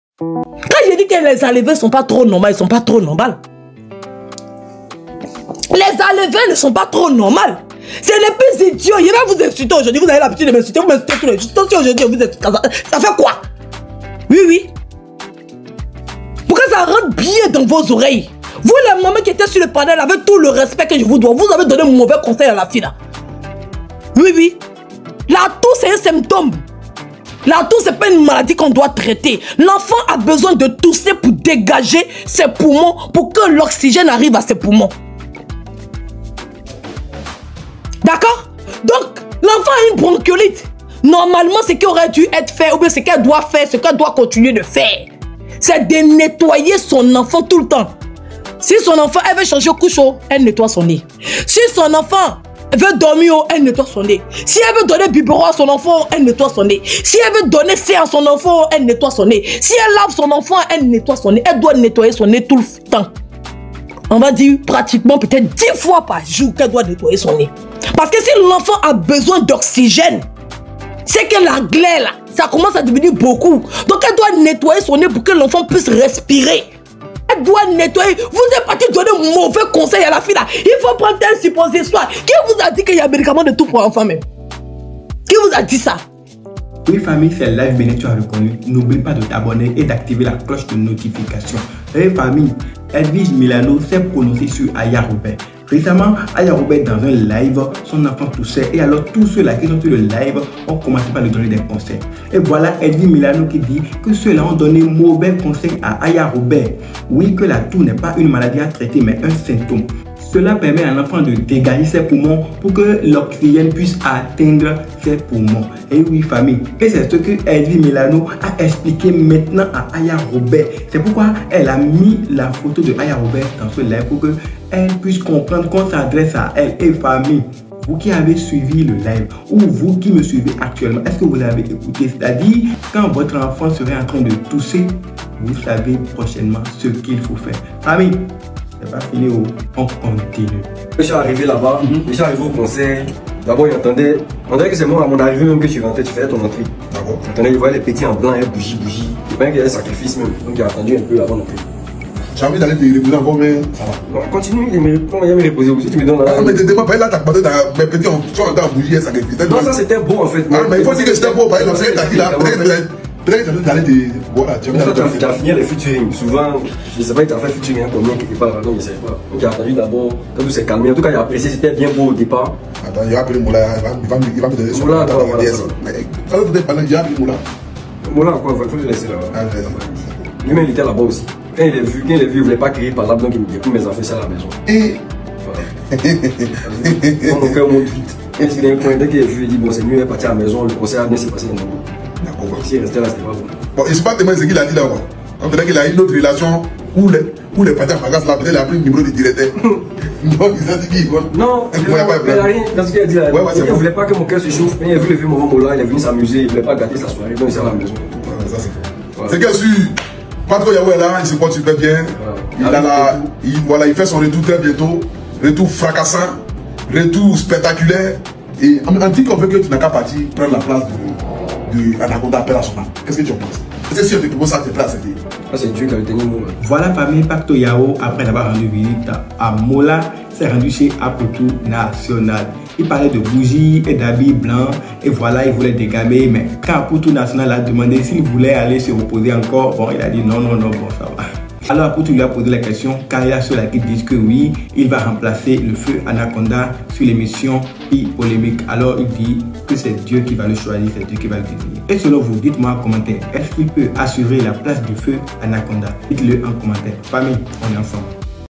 🚨 BUZZ DU JOUR : APOUTCHOU NATIONAL, PATCKO YAO, AYA ROBERT, EDWIGE MILAN
Nous avons décidé aujourd'hui de vous promener dans les coulisses du buzz. Suivez-nous pour tout savoir sur la rencontre entre Apoutchou National et Patcko Yao, l'affaire Aya Robert, et tout sur Edwige Milan.

Rejoignez-nous sur notre page Facebook où nous partageons l'actualité dans le sourire 😄

Nous vous proposons aussi nos trois dernières vidéos de la chaîne.